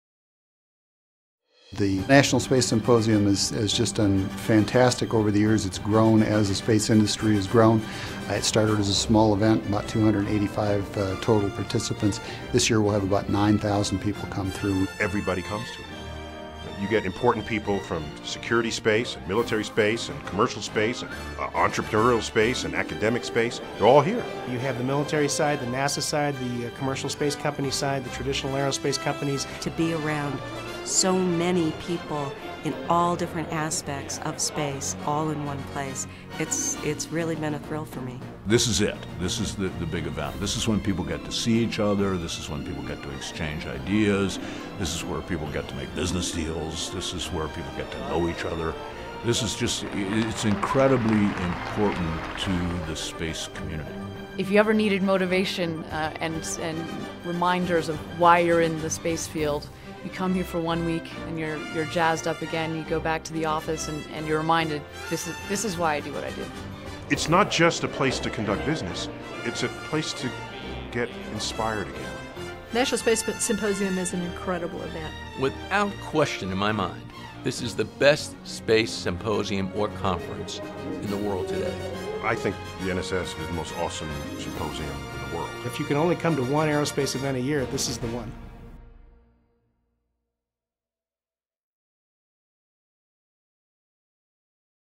National Space Symposium
The National Space Symposium is the premier gathering of the global space community. Hear what industry leaders have to say about it.